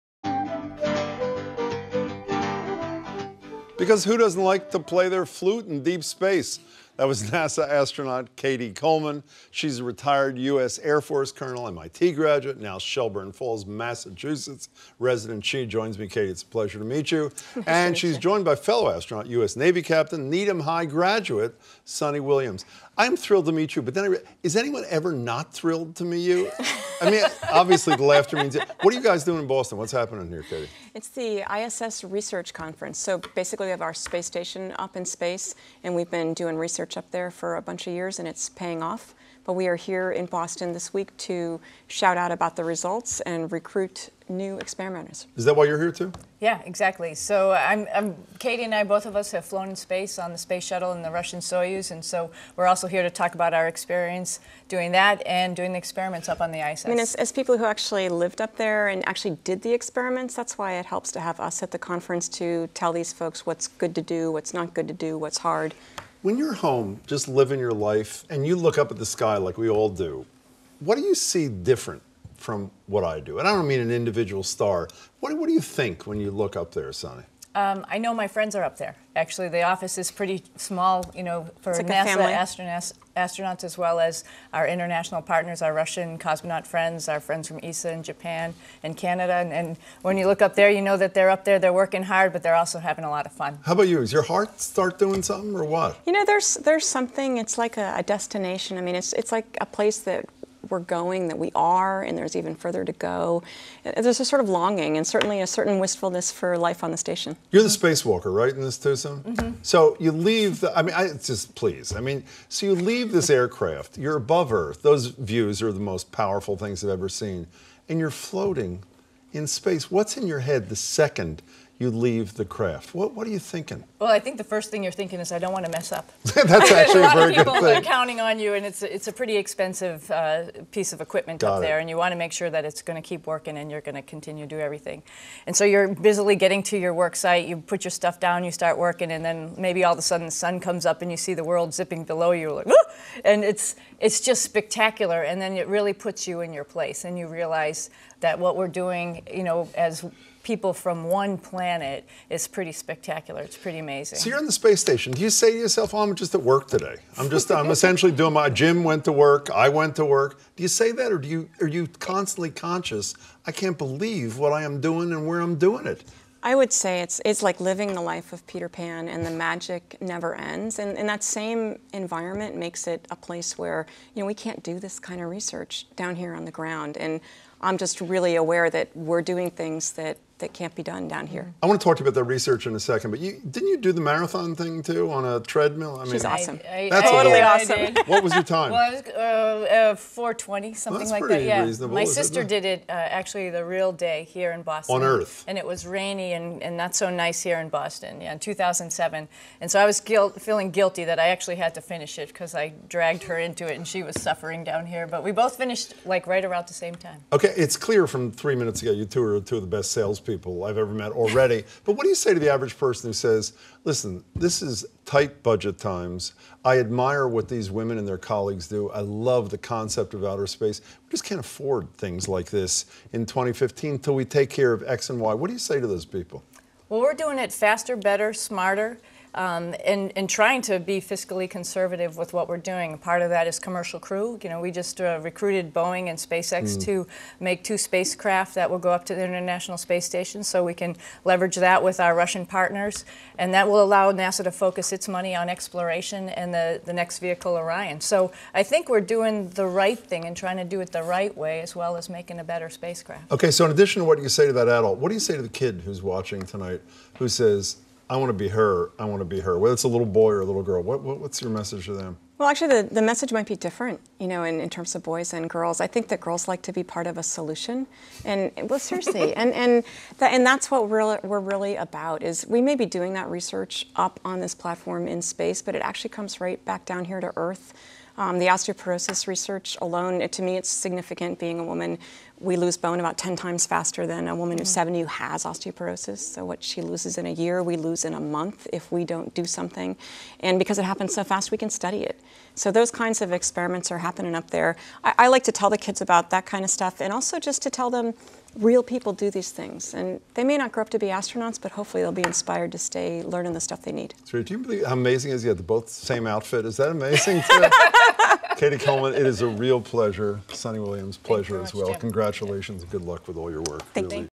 WATCH: NASA Astronauts Talk Space Station
NASA Astronauts Cady Coleman (@Astro_Cady) and Suni Williams (@Astro_Suni) discuss their time on the space station.